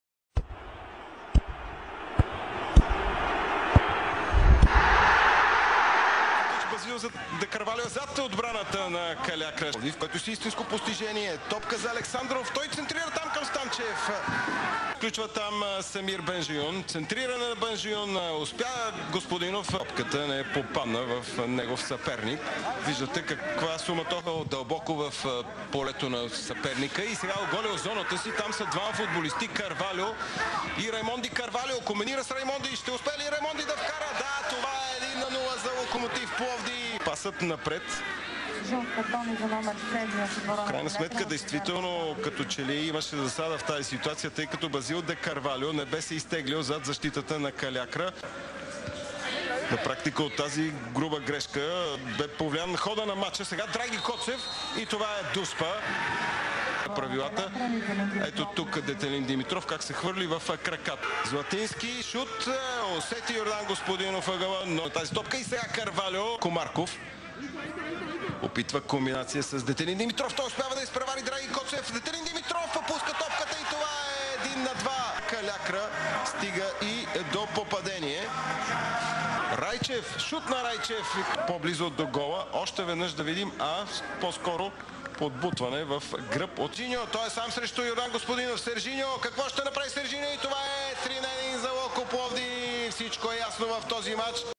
25.04.2011 Kaliakra Kavarna - Lokomotiv Plovdiv 1:3
Kaliakra Kavarna - Lokomotiv Plovdiv 1:3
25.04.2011Goal scored: Raymondi, Zlatinski, Serginhyo